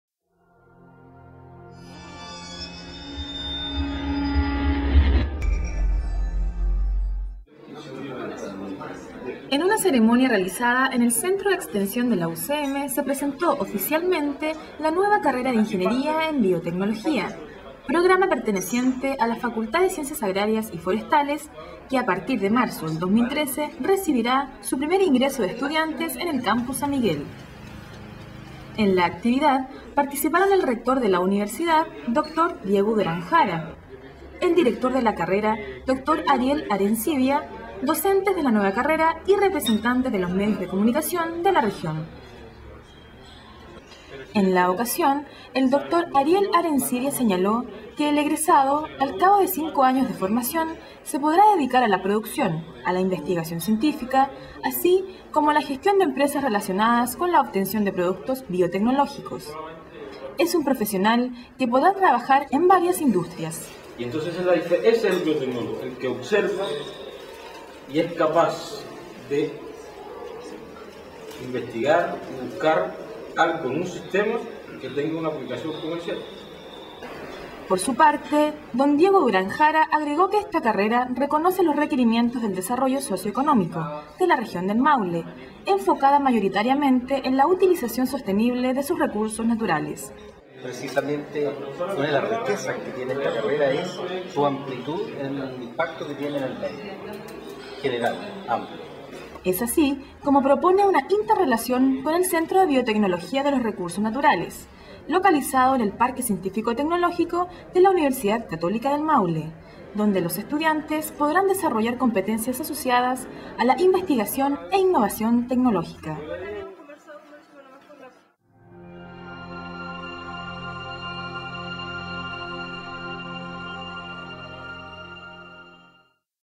Nueva Carrera Ingeniería en Biotecnología UCM
Nueva carrera UCM Talca: Ingeniería en Biotecnología
El rector Dr. Diego Durán señaló que la carrera "es un avance para la región"

En una ceremonia realizada en el Centro de Extensión de nuestra, se presentó oficialmente a la región y el país la nueva carrera de Ingeniería en Biotecnología, programa perteneciente a la Facultad de Ciencias Agrarias y Forestales, que a partir de marzo de 2013 recibirá su primer ingreso de estudiantes en el Campus San Miguel.